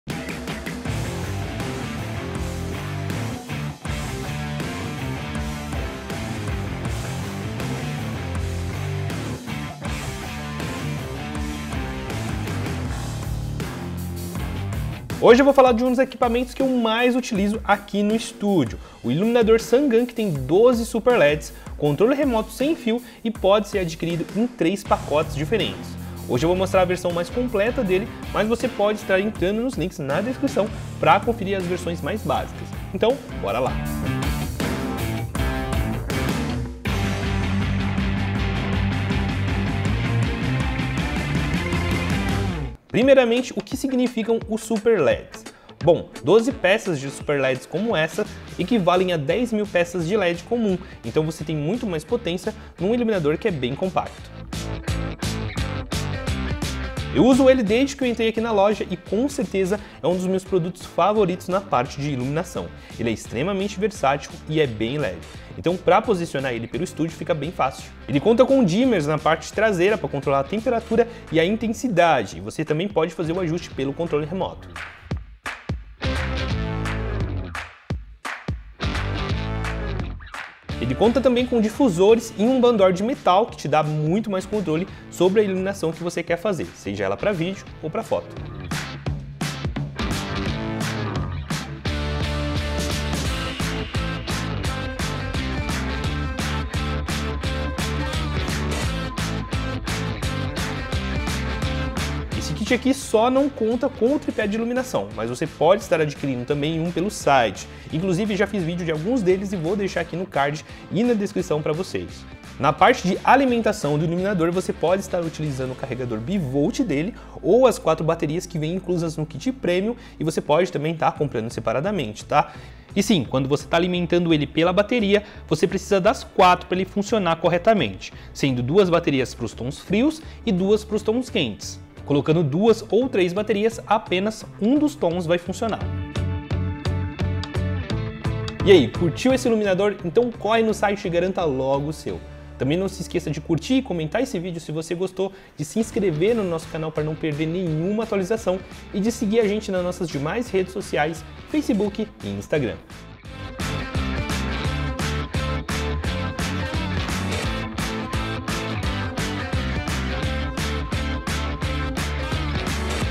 🔴 Sun Gun WV-10000 para iluminação - O poder incrível de 12 SUPER LEDS!
📣Não invista em iluminação sem conhecer o nosso sun gun com 12 super leds

O Iluminador Sun Gun de 12 Super LEDs com Controle Remoto Sem Fio, case rígido e Fonte (110-220v). A luz ambiente nem sempre é o necessário para que você tenha qualidade de imagem e iluminação, principalmente em Estúdios Fotográficos. Para fazer a compensação de luz durante entrevistas, ao ar livre ou em mini estúdios fotográficos muitos fotógrafos utilizam diversos dispositivos.

A potência de Iluminador de 12 Super LEDs com Controle Sem Fio (110-220v) pode ser controlada durante o uso através de controle remoto sem fio, que permite ajustar não somente a intensidade, mas também a temperatura de cor entre 5600k até 3200k, podendo ser mudada a qualquer momento com os difusores inclusos no kit para suavizar a luz dura, criando tons mais quentes, mais frios ou balanceiam para maior naturalidade.Os Bandoors laterais ajudam a direcionar a luz para o motivo evitando dispersão e sombra, suas dobradiças são de metal e possuem movimento suave.

Este Iluminador Sun Gun de 12 Super LEDs com Controle Remoto Sem Fio, case rígido e Fonte (110-220v). É ainda mais compacto, prático e simples de se usar. Ele possui sapata padrão para tripés de iluminação, podendo ser usado em estúdios profissionais, mini estúdios, entrevistas e diversos tipos de gravação. Ele também pode ser utilizado por até 5 horas consecutivas com máximo de potência com as 4 baterias (não inclusas) em seus encaixes na parte de trás de sua estrutura, que alcançam até 43.000 lumiens, podendo ser ajustado manualmente através dos dimmers ou do controle remoto sem fio.

Além disso, nesse kit está incluso um case rígido para transporte de equipamentos com espuma Pré cortada para fornecer segurança e conforto durante viagens. O case é à prova de água, altas temperaturas e fortes impactos.

📍Se você gostou do poder incrível desses 12 Super Leds, iremos deixar na descrição onde adquirir o seu, só acessar o link abaixo, você vai encontrar esse e outros modelos mais básicos . Adquira já o seu !👇

⚫Videos Sugeridos sobre tripés